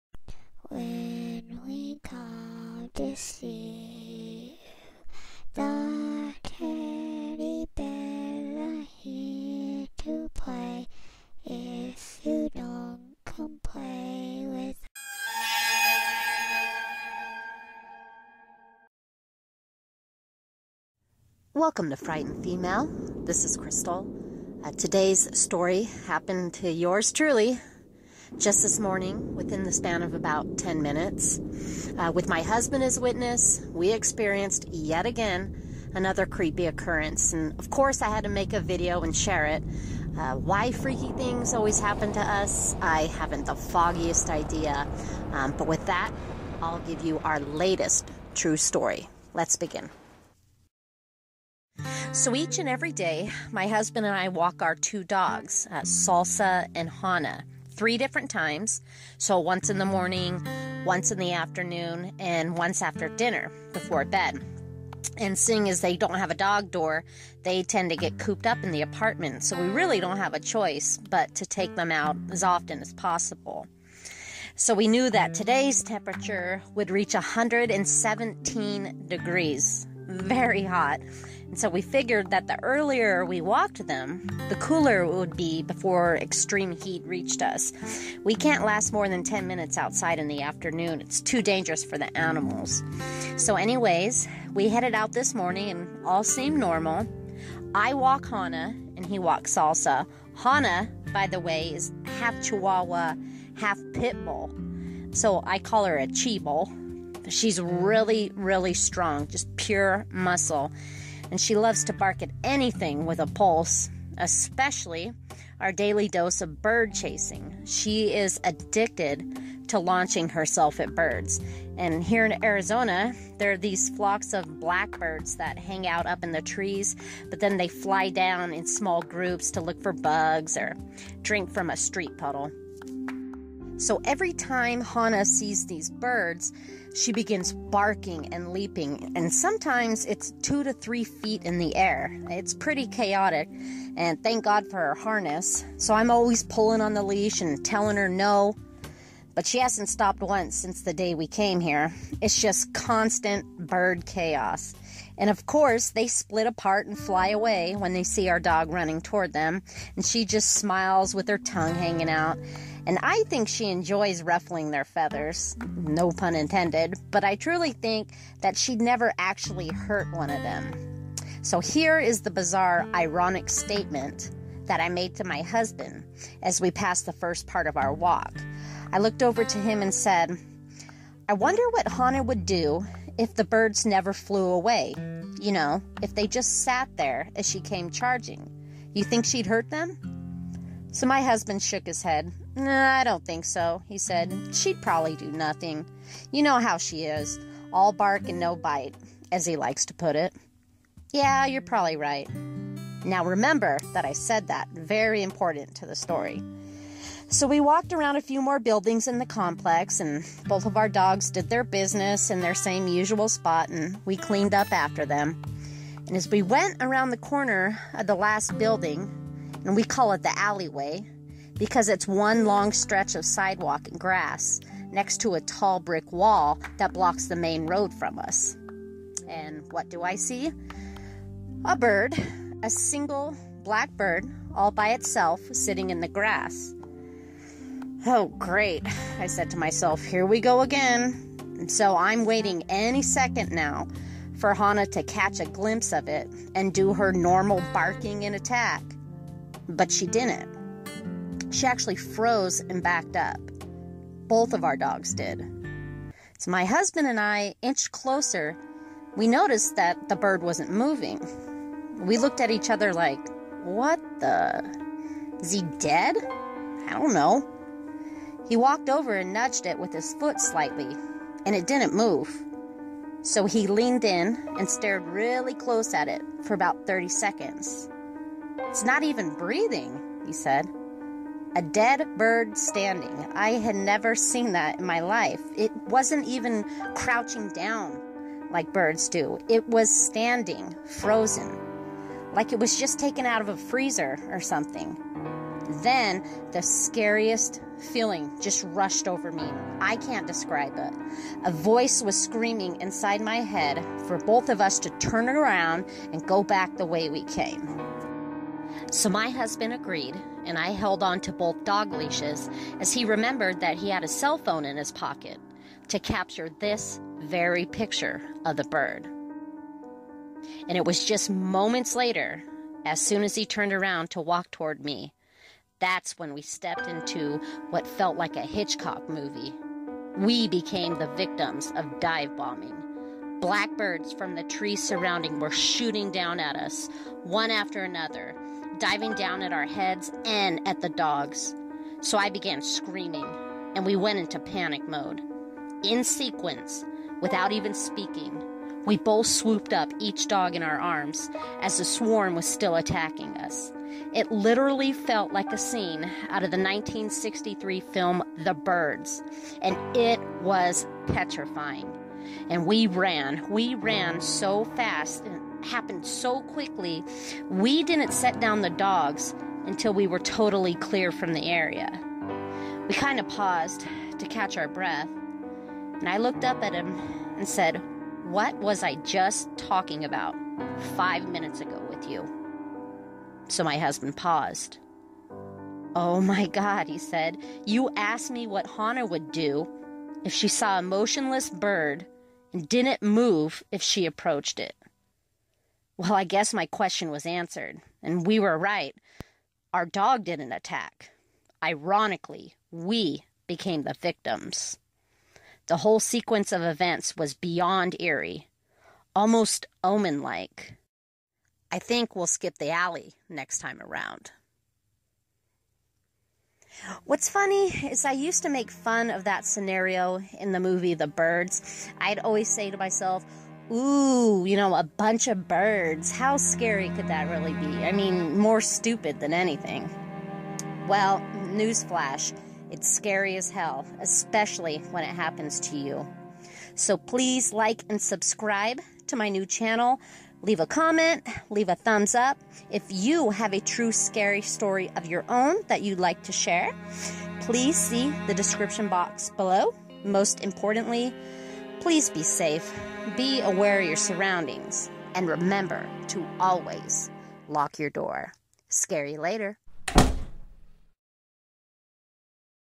TRUE Creepy Story That Just Happened To Me - The Blackbird Omen
Just another day of walking the dogs and fighting the desert heat, right? But little did we know that the subject matter (brought up by yours truly) would unfold into reality with a bizarre ending, causing us to run like hell in the course of 10 minutes!

If you have a true scary story of your own that you'd like to submit, please email it to me:

(Please include your first name or user name so that I can give you credit in an upcoming video!)